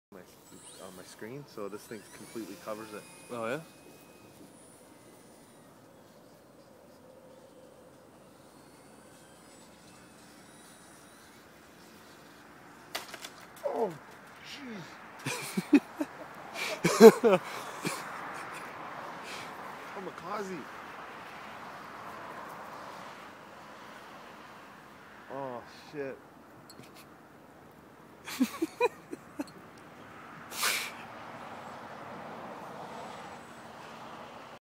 Barnstorming Beast crashes into the barn.
The pilot, Spike had been doing several successful passes prior to this incident. It seems that whenever the camera comes out a crash is sure to be captured.